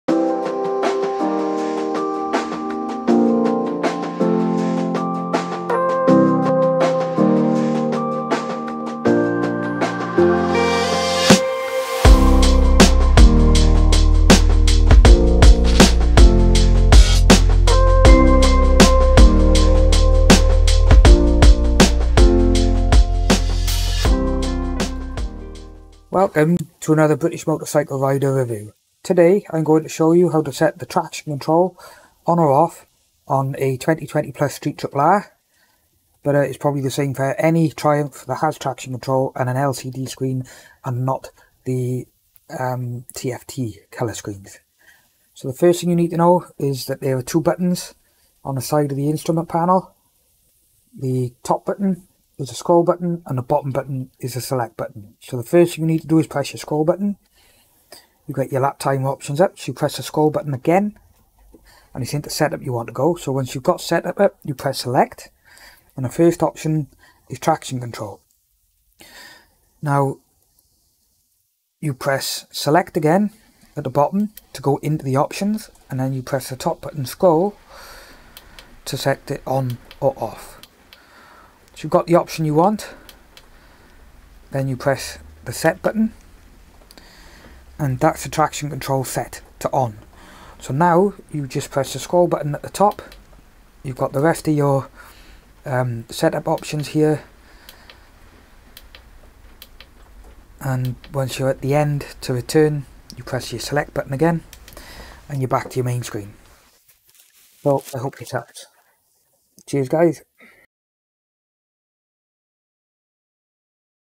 Turning off traction control on Street Triple R
How to turn the traction control on and off on a 2020+ Street Triple R (LCD screen)